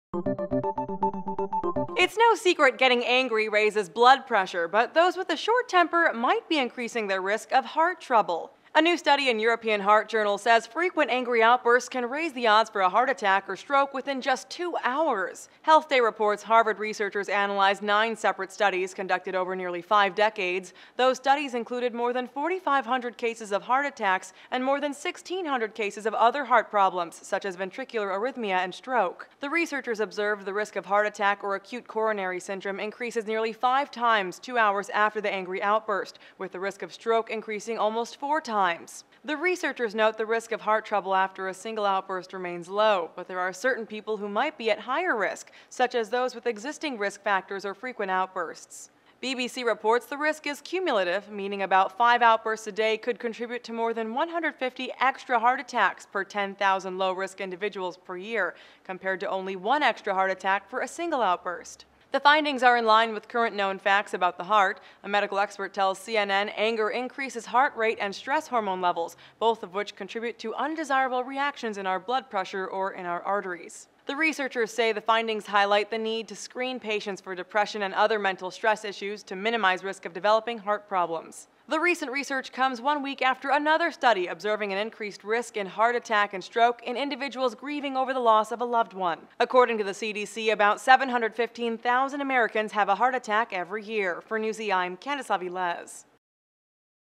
How Your Temper Could Affect Your Heart
Researchers observed an increased risk in various heart-related problems including stroke and heart attack in people who frequently get angry.